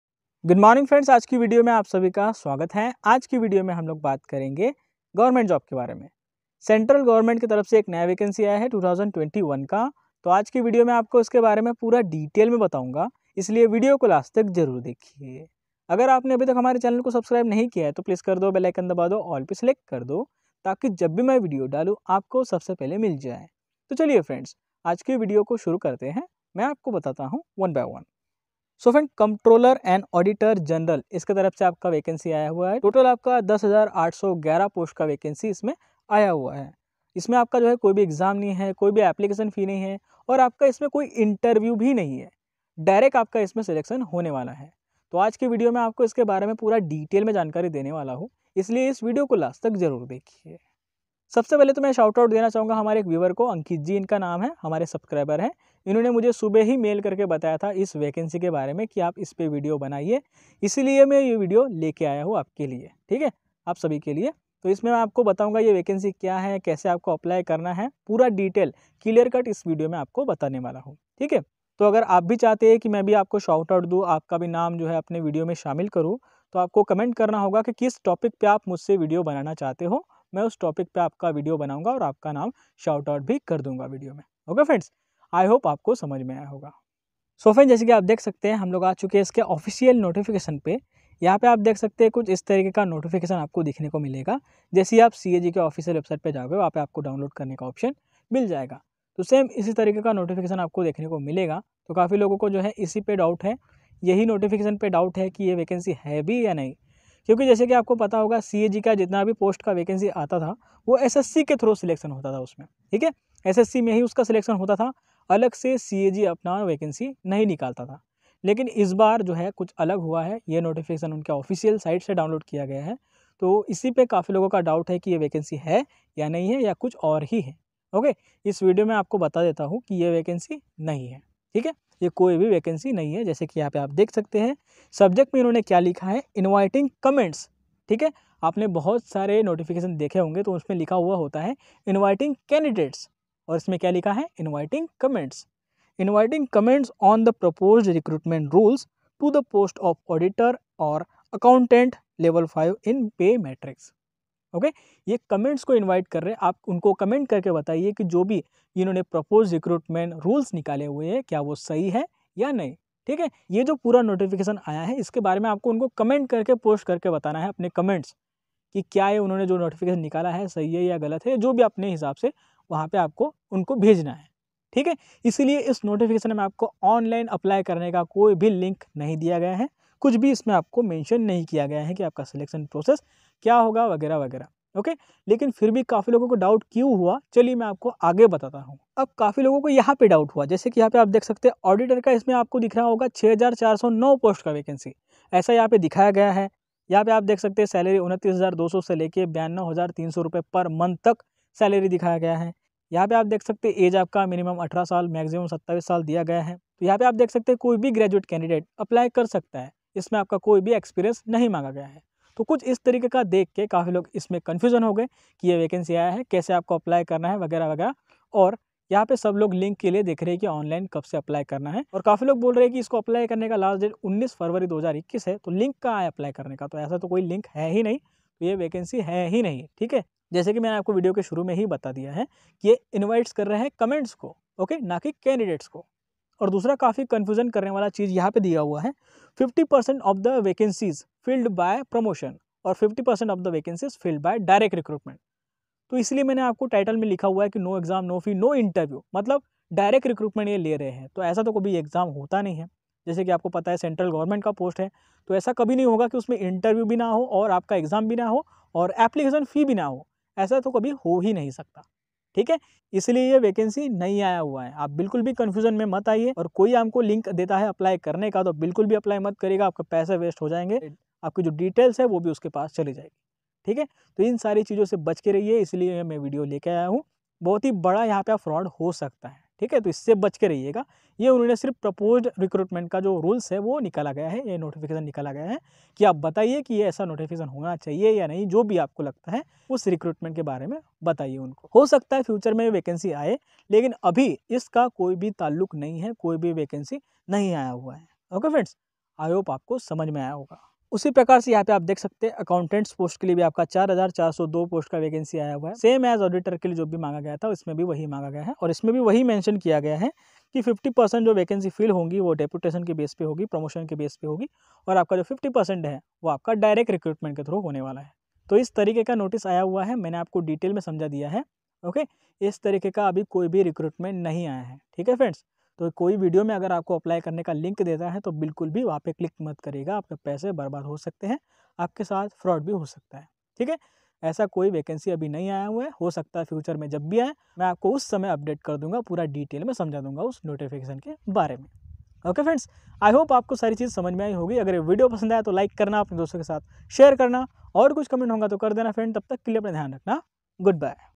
Central Govt Job Recruitment 2021 | 10,811 Post | No Exam | No Fee | No Interview | Direct Selection
In this video I have discussed about Central Govt Job Recruitment 2021.

Telegram Group Name :- @youngerage73

Central Govt Job Recruitment 2021

CAG Recruitment 2021.

cag 2021